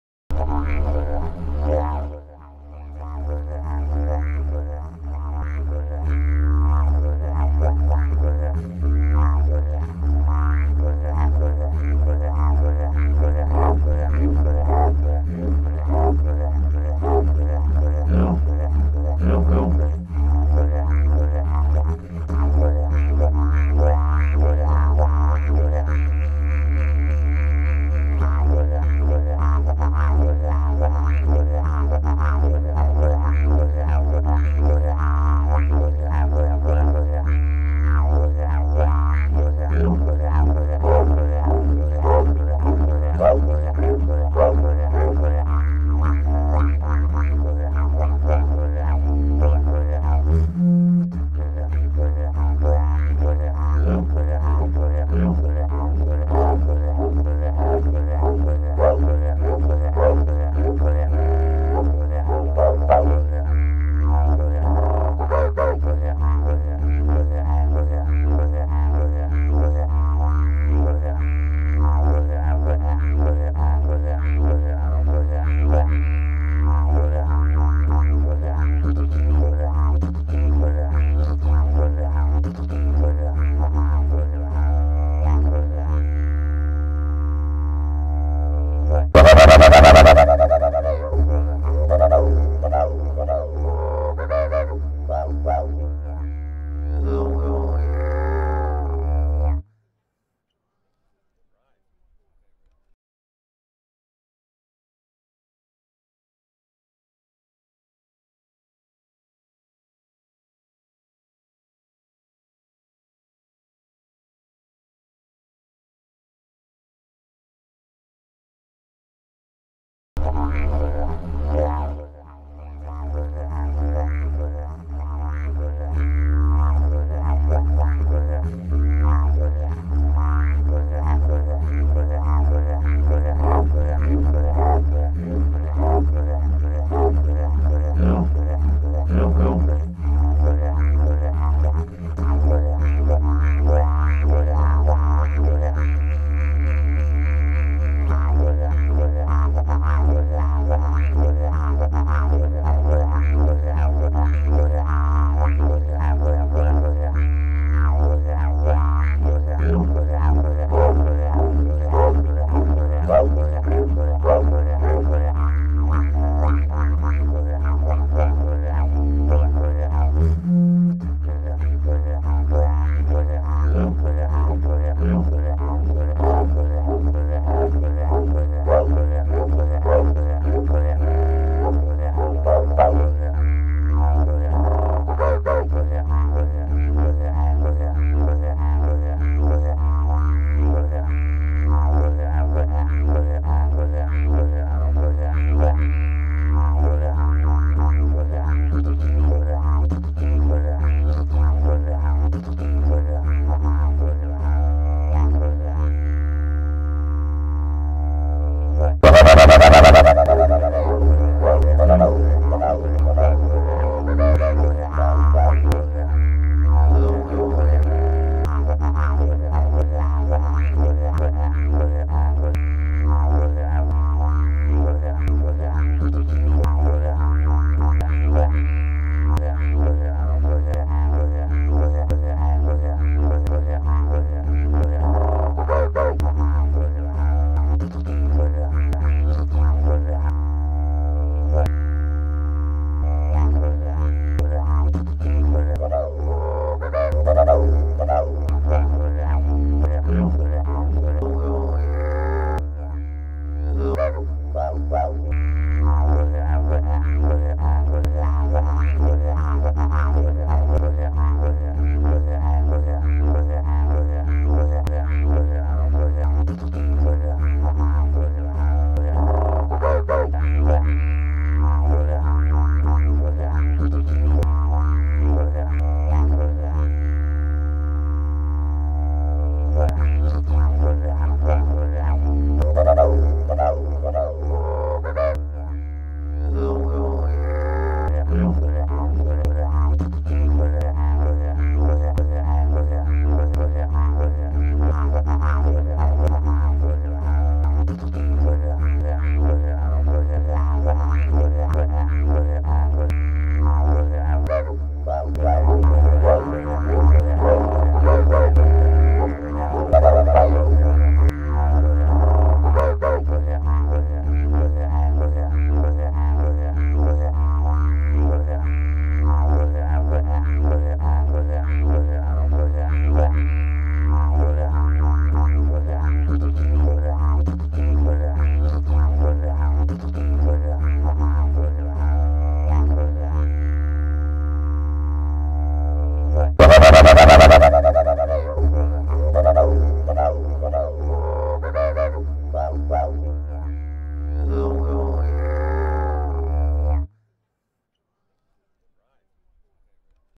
digerdoo danny part-2 union sq park nyc 7-10-09
music from digerdoo danny part-2 recorded at union sq park nyc 7-10-09 - part 1 -& 3 danny digerdoo insightful conversation about his relationship to the digeridoo and music and life too